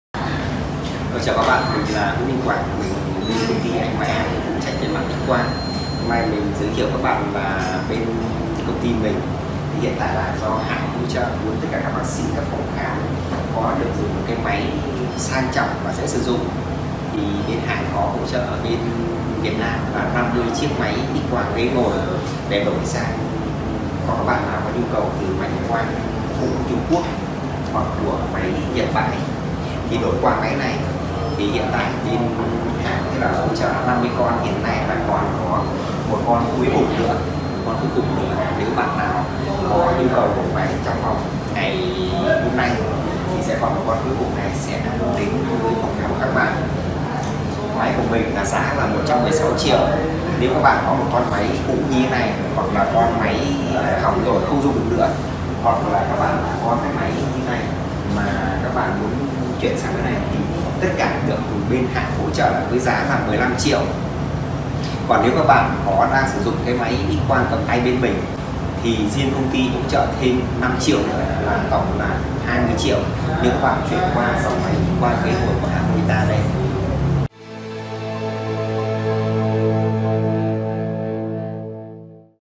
Máy xquang nha khoa nhật xin J. Morita
TẠI SAO BẠN NÊN ĐẦU TƯ X-QUANG MORITA❔❔

Là một nha khoa bạn cần có máy x-quang, và bạn nên chọn x-quang Morita, bởi vì sao?

🍀 X-quang Morita cho hình ảnh rõ nét giúp bạn dễ dàng chẩn đoán tình trạng của bệnh nhân, cải thiện được bệnh tình của bệnh nhân tốt hơn tạo được sự tin cậy và truyền bá hình ảnh phòng khám tới khách hàng khác tốt nhất, khách hàng sẽ xem phòng khám của bạn là sự lựa chọn hàng đầu cho bản thân và gia đình.
🍀 Nâng tầm đẳng cấp và độ chuyên nghiệp của phòng khám nhờ thiết kế ghế ngồi sang trọng.
Máy có độ bền cao và không gặp nguy hiểm về cháy nổ pin như X-quang cầm tay.